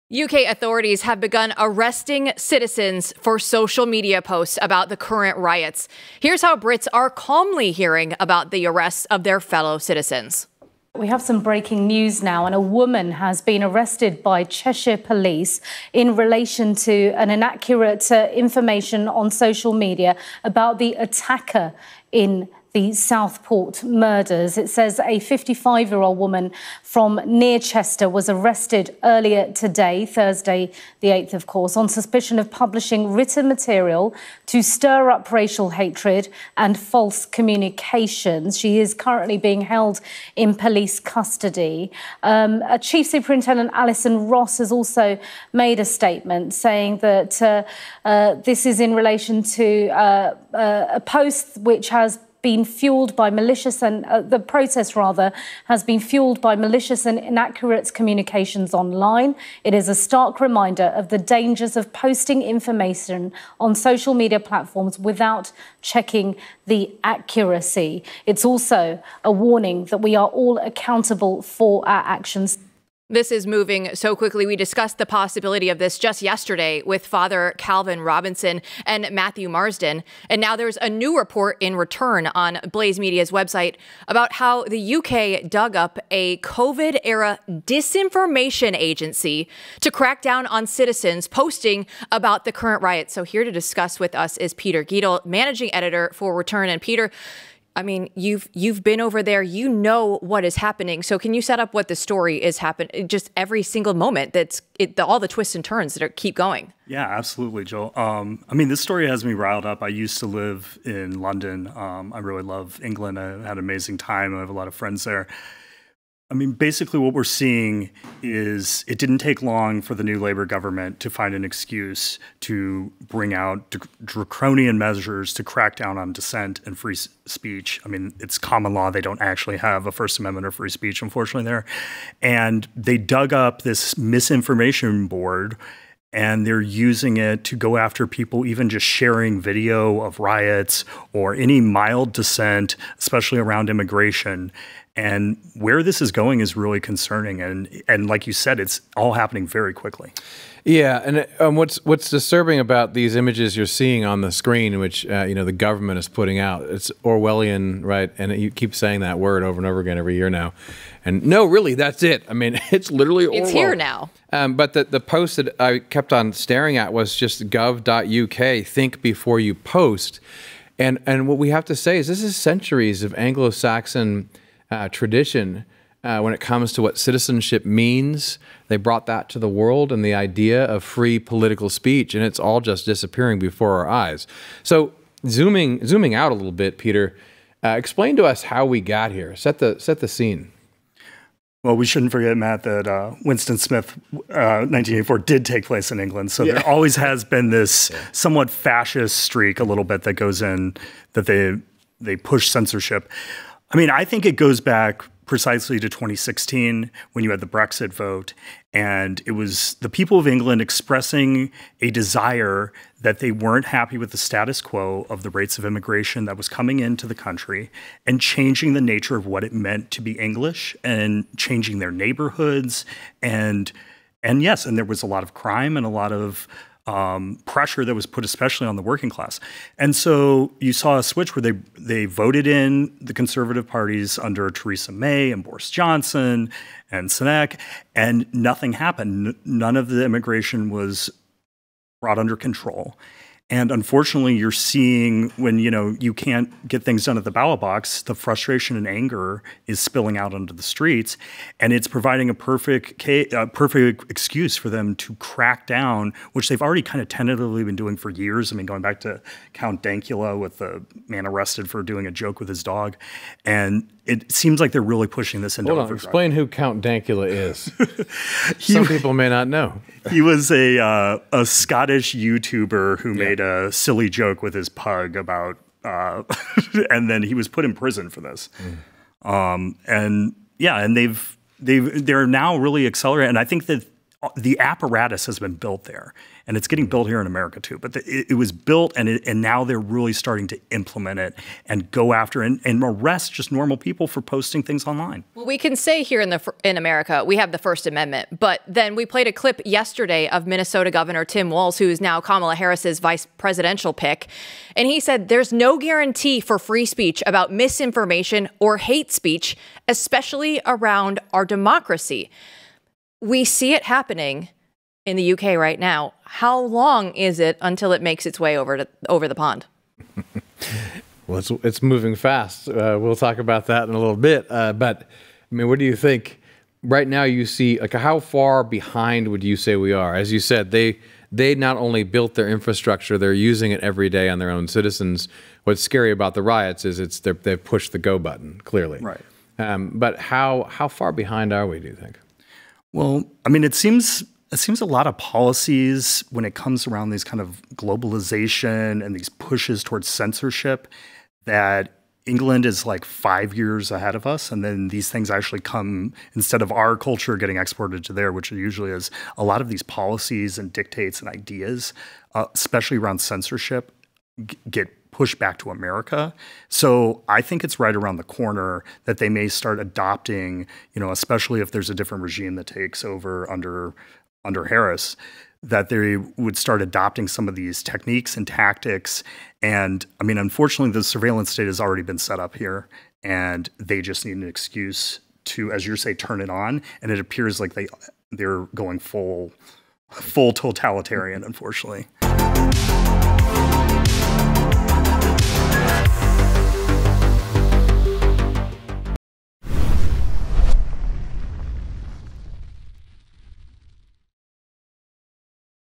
England Goes FULL ORWELL | UK Gov't JAILS Citizens for Social Media Posts!
The United Kingdom has resurrected a Covid era measure to censor law abiding citizens. Peter Gietl, the Managing Editor for Blaze Media's Return, explains how the British Government is censoring dissent from the "far-right" while letting leftists harass Britons without consequences. Gietl warns the United States is only five years behind Great Britain in terms of censorship.